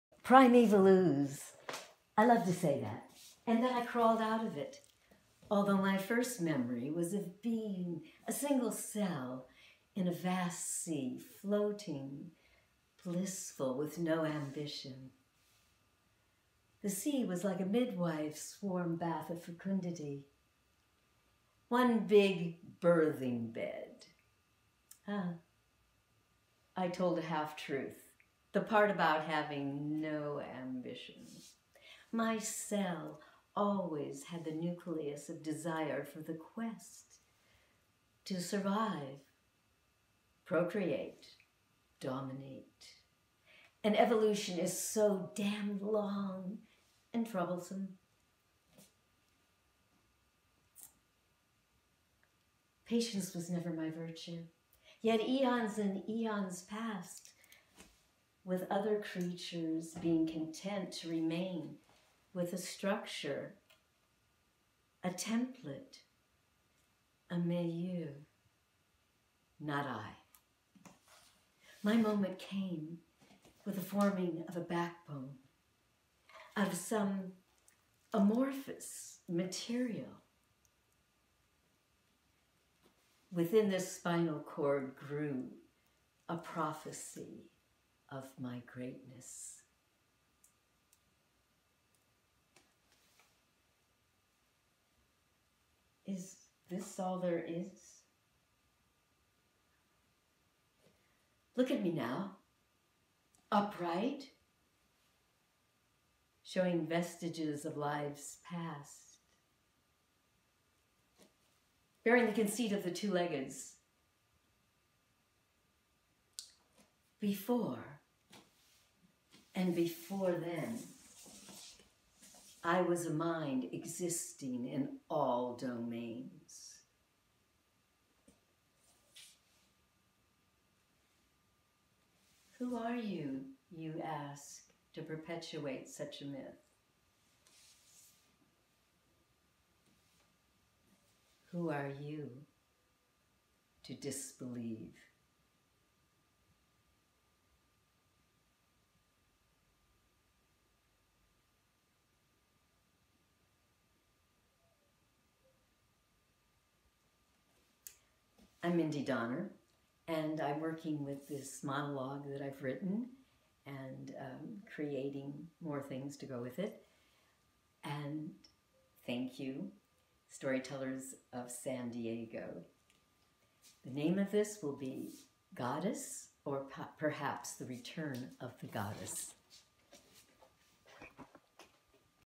Goddess Returns
Mindy Donner spins an original tale of wonder and transformation for the Storytellers of San Diego, June 2020 virtual concert: "Time Keeps Traveling On," for Eclipse Chocolate Bar & Bistro